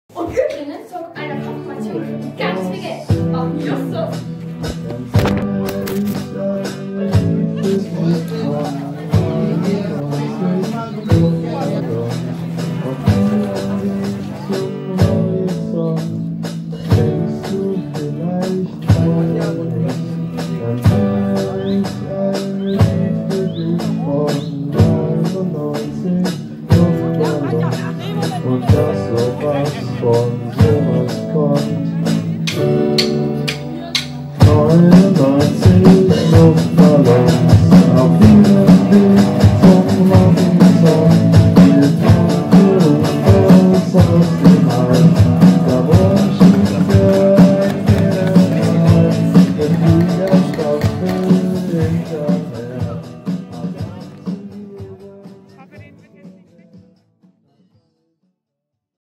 KonfiCamp am Arendsee
154 Konfis, 51 Teamer:innen, zwei Handvoll Hauptberufliche und mehr als 20 Workshops - so sah das KonfiCamp 2022 in Zahlen aus. Fünf Reisebusse sind zum Arendsee in der Altmark gefahren, endlich wieder nach zwei Jahren Corona-Pause.